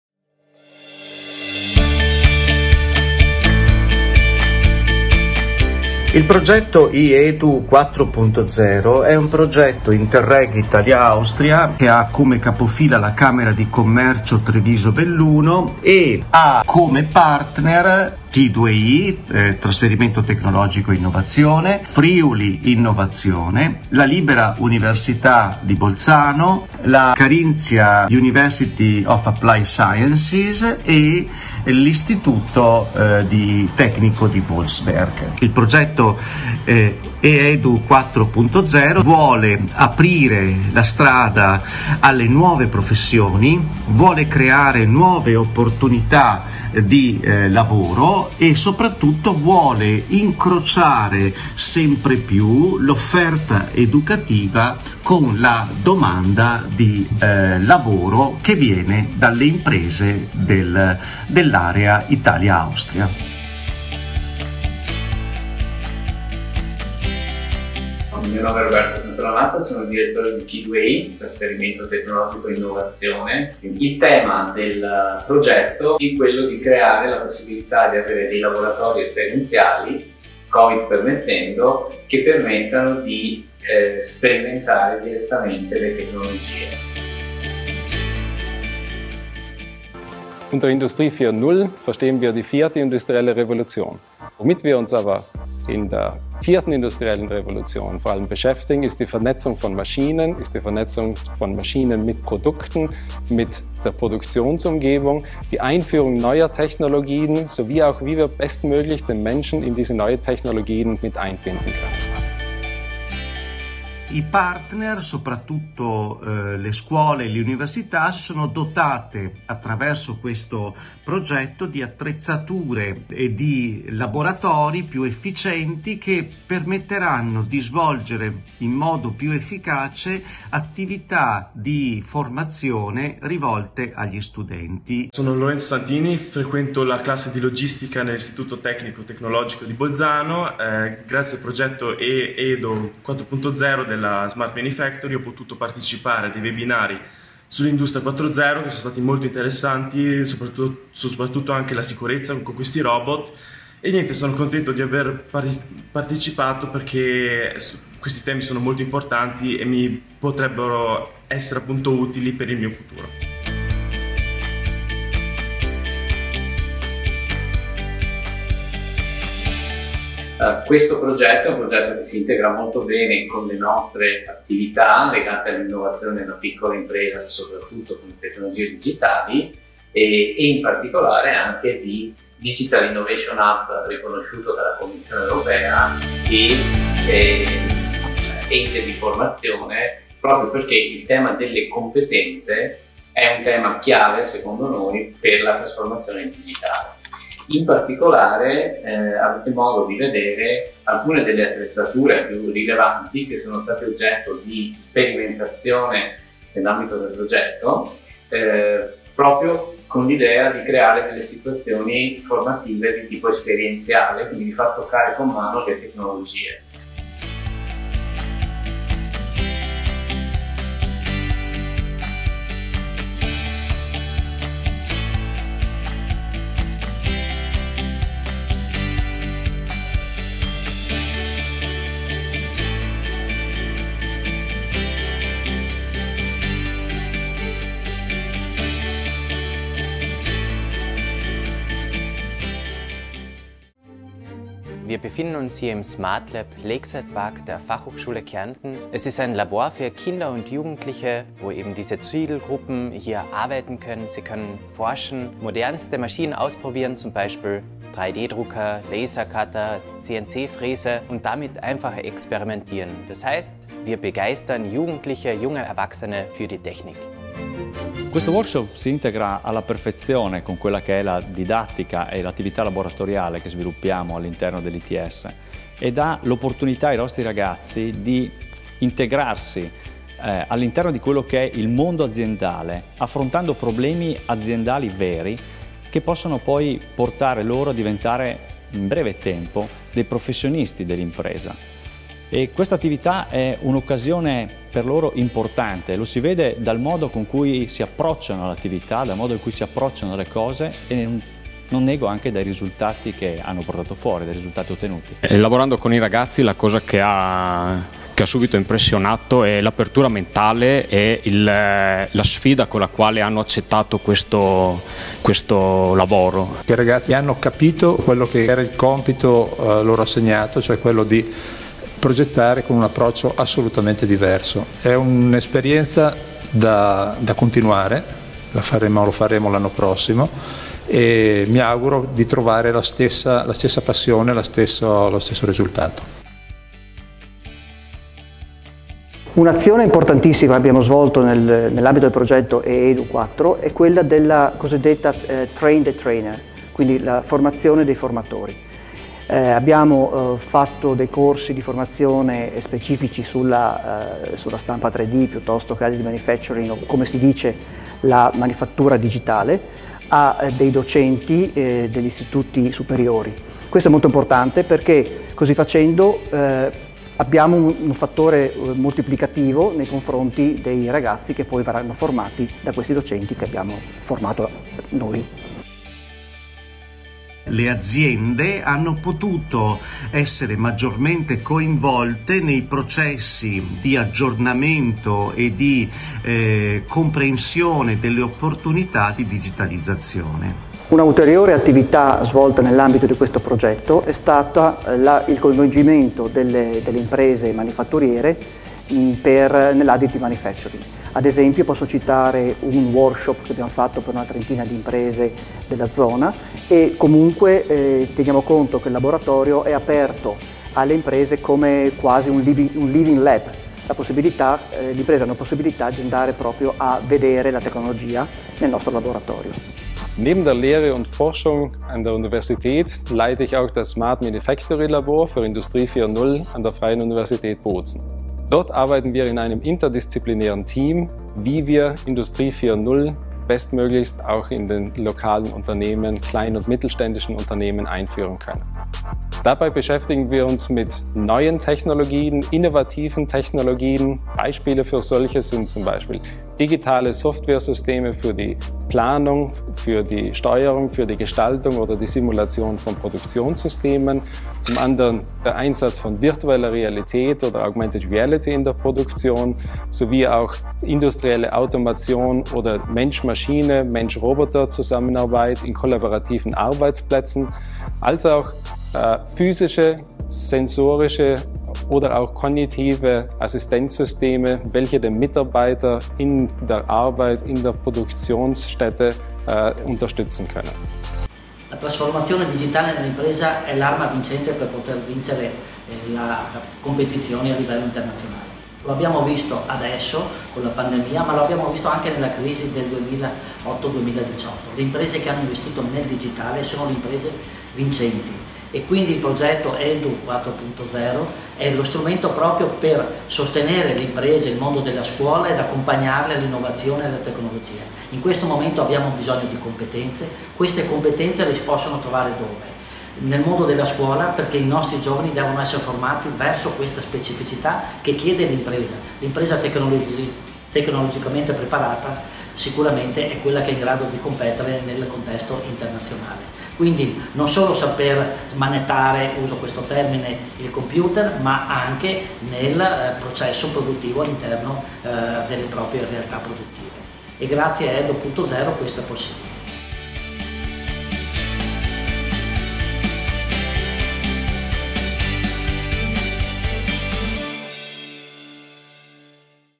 Video presentazione del Progetto E-Edu 4.0
Il progetto E-EDU 4.0 sostiene Industria 4.0 rafforzando l'impatto dei programmi di formazione e di istruzione con una cooperazione transfrontaliera molto più efficace e una migliore sinergia da un lato con i programmi di formazione e didattica e dall’altra tra laboratori e infrastrutture didattiche.
Il progetto darà vita a quelle necessarie reti di attori che hanno competenze nei programmi di formazione, svolgono attività e forniscono gli strumenti e le attrezzature necessarie per rafforzare i processi di apprendimento sia a livello locale che transnazionale.
La sperimentazione concreta dei programmi di qualificazione con azioni locali e transfrontaliere destinate alle PMI e agli studenti migliorerà l'impatto del progetto e permetterà nuove sinergie e nuove applicazioni. Una piattaforma ICT con contenuti didattici e una mappatura delle competenze supporterà le reti, la cooperazione e la sostenibilità delle azioni intraprese nel quadro del progetto.

L'obiettivo del progetto E-EDU 4.0 è sviluppare una rete di qualificazione transfrontaliera, un programma nonché un'offerta di qualificazione condivisa nell’area di programma.
Il progetto E-EDU 4.0 si propone di armonizzare i programmi educativi attraverso:
- la creazione di un network transfrontaliero degli operatori della formazione e delle infrastrutture disponibili;
- la definizione di modelli di cooperazione per migliorare l’offerta di formazione qualificata per studenti e lavoratori;
- l’allestimento di laboratori per svolgere attività didattiche;
- l’elaborazione di un programma di studio per la qualifica professionale di insegnanti, studenti, lavoratori ed imprenditori sui temi relativi ad Industria 4.0;
- l’attivazione di corsi di formazione rivolti a studenti, giovani imprenditori e “talenti digitali”.

Partner:
Camera di Commercio Industria artigianato agricoltura di Treviso Belluno (coordinatore), HTL Wolfsberg, Carinthia Universtiy for Applied Sciences, t2i – trasferimento tecnologico e innovazione s.c. a r.l., Libera Università di Bolzano, Friuli Innovazione Centro di Ricerca e Trasferimento Tecnologico, Italia